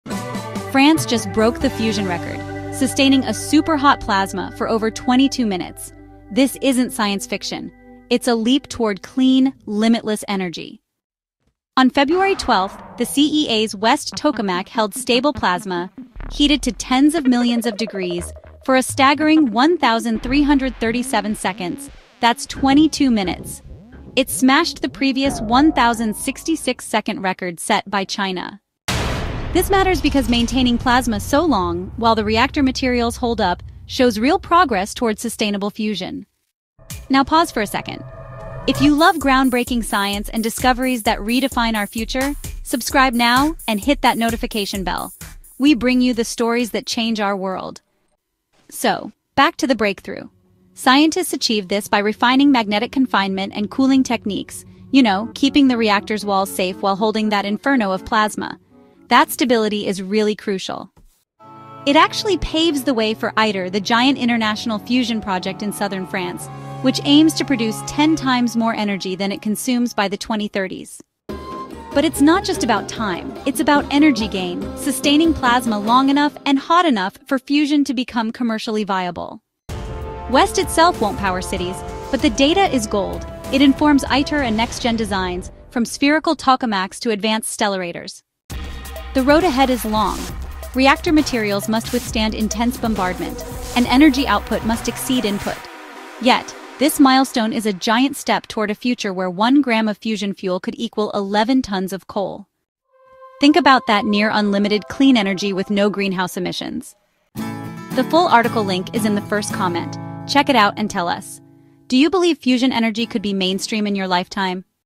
France’s Fusion Reactor SMASHES Record—22 Minutes of Super‑Hot Plasma!
France's WEST Tokamak just set a new fusion record—22 minutes of stable plasma!
This breakthrough brings us one giant step closer to clean, virtually limitless energy.
Want to know how it was done—and what’s next for ITER?

📌 Full article linked in the first comment